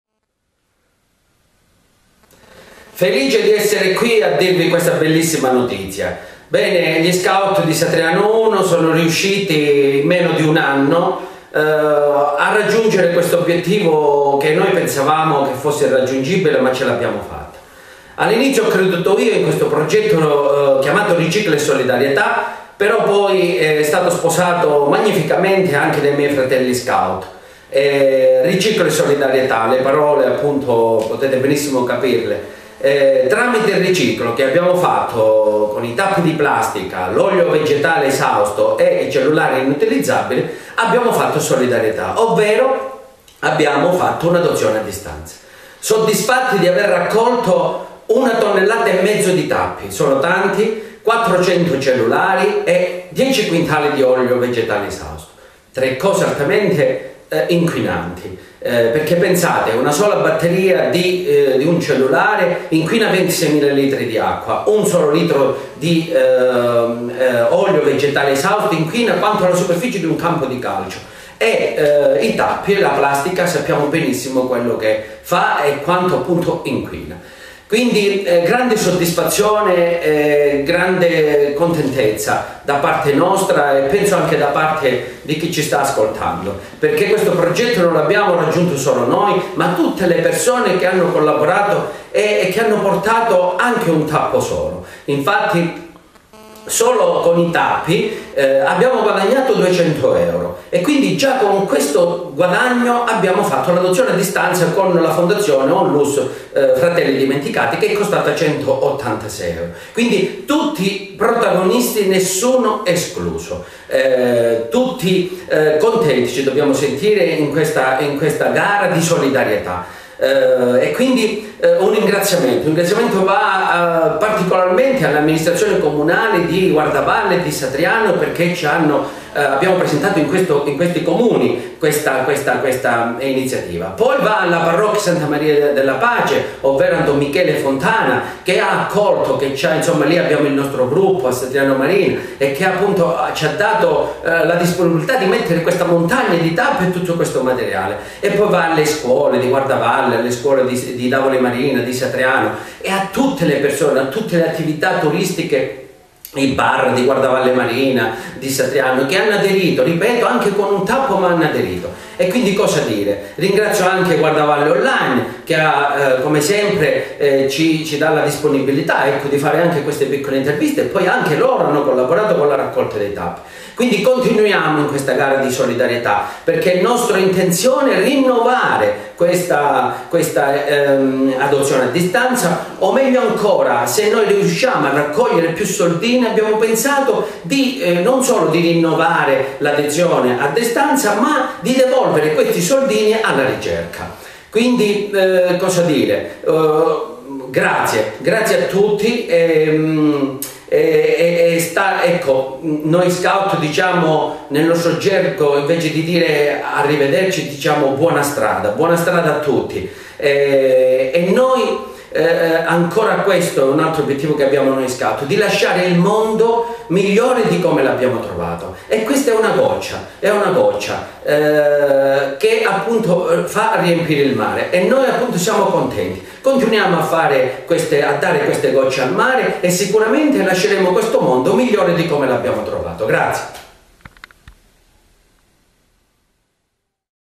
Guardavalle: Giovanni Montepaone ringrazia per il successo della raccolta tappi di plastica
Nel video racconta la soddisfazione del raggiungimento di 1500kg di plastica.